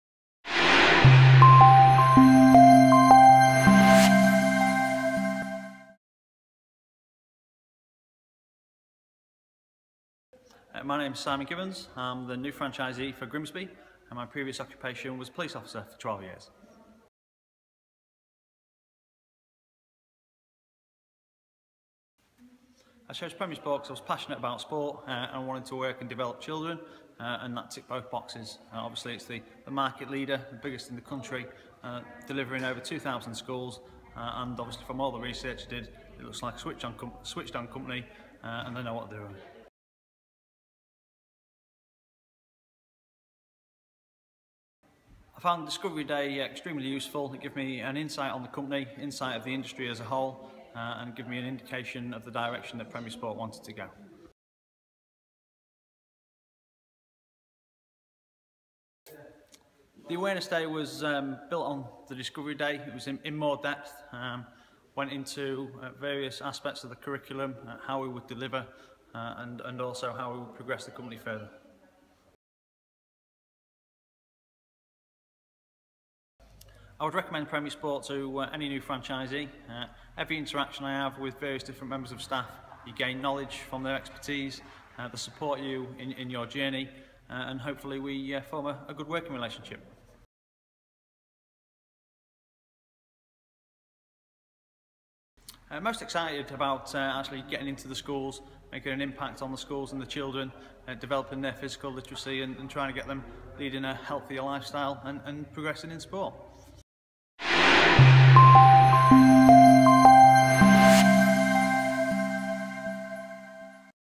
Introducing Franchisee Simon Gibbons - Grimsby
New franchisee Simon Gibbons shares his story.